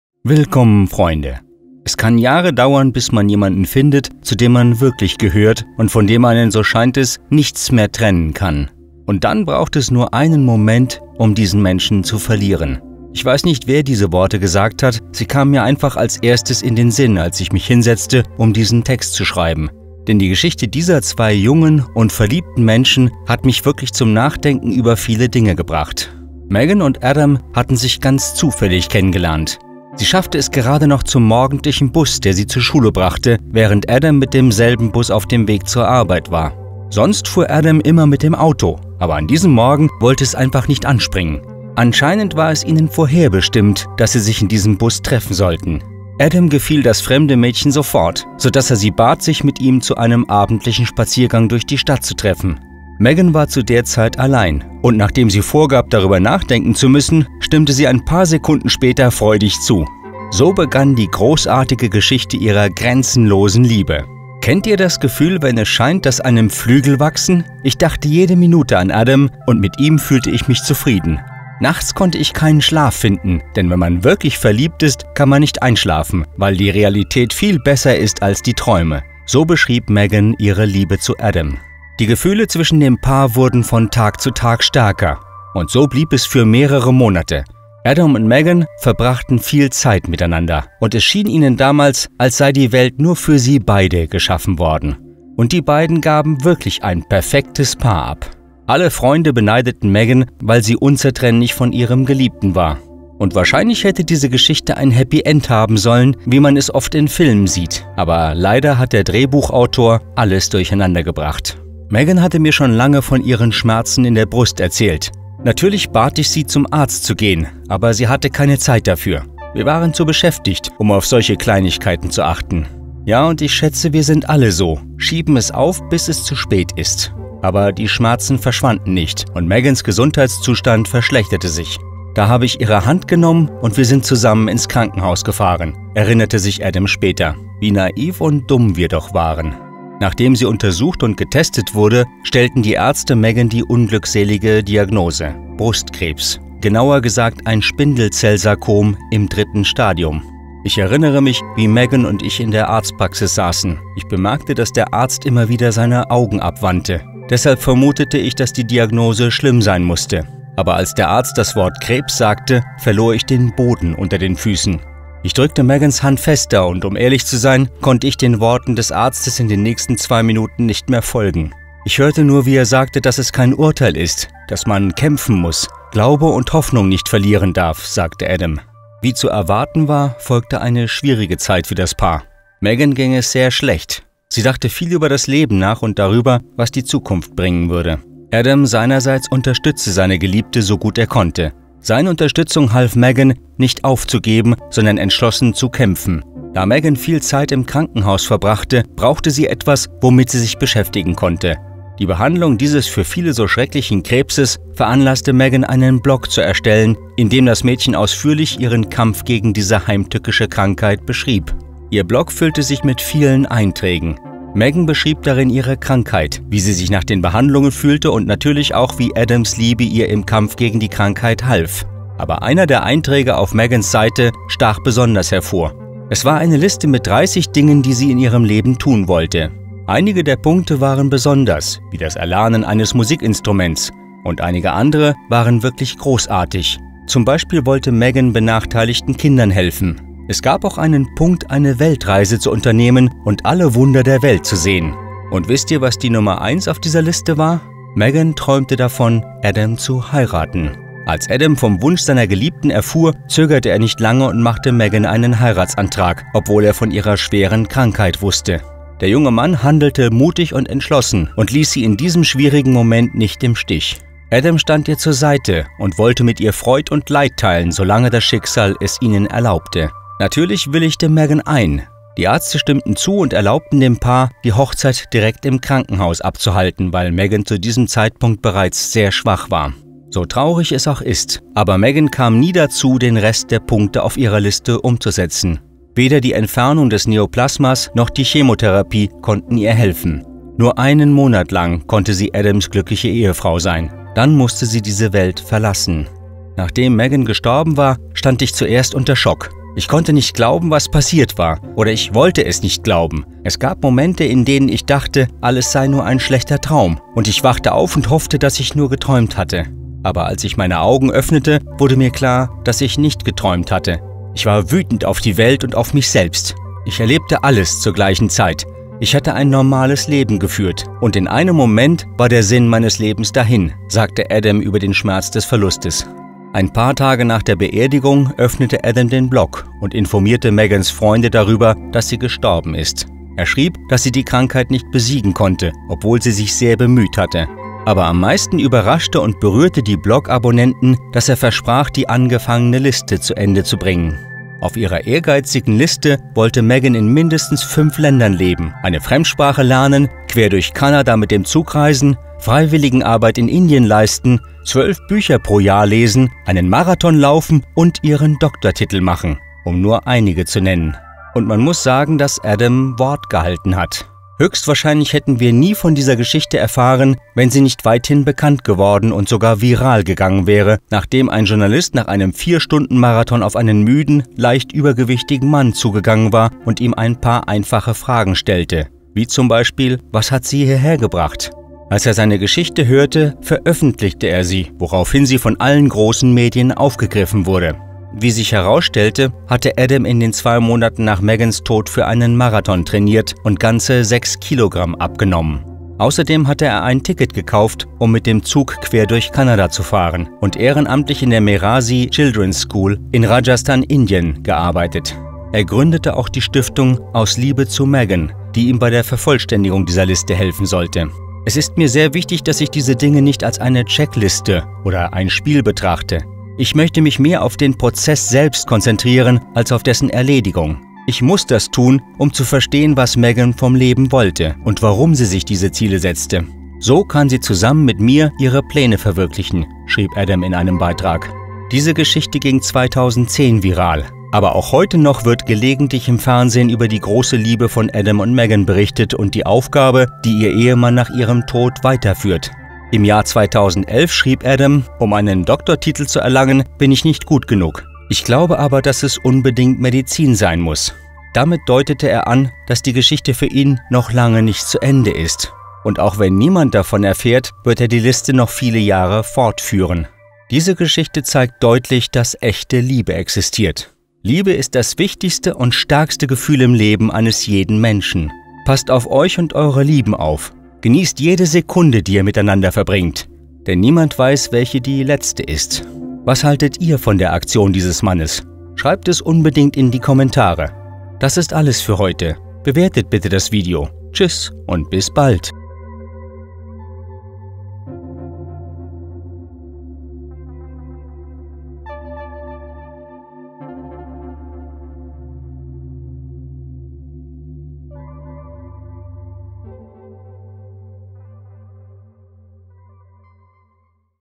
Er verlor sie einen Monat nach der HOCHZEIT... Was der Bräutigam getan hat, ist SCHOCKIEREND ...
Es kann Jahre dauern, bis man jemanden findet, zu dem man wirklich gehört und von dem einen, so scheint es, nichts mehr trennen kann. Und dann braucht es nur einen Augenblick, um diesen Menschen zu verlieren ... Ich weiß nicht, wer diese Worte gesagt hat, sie kamen mir einfach als erstes in den Sinn, als ich mich hinsetzte, um diesen Text zu schreiben ...